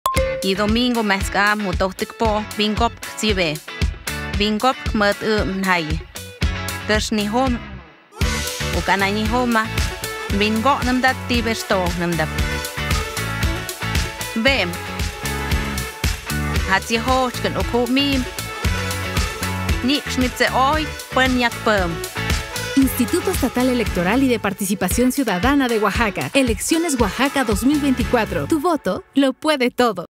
Promoción del voto | Elecciones Oaxaca 2024 - Mixe
🗳️Este domingo 2dejunio, será un día grande para democracia en Oaxaca
🙋🏽‍♀️🙋🏽‍♂️Tu participación y tu voto son muy importantes. y se parte de las EleccionesOaxaca2024; no lo olvides, TuVotoLoPuedeTodo.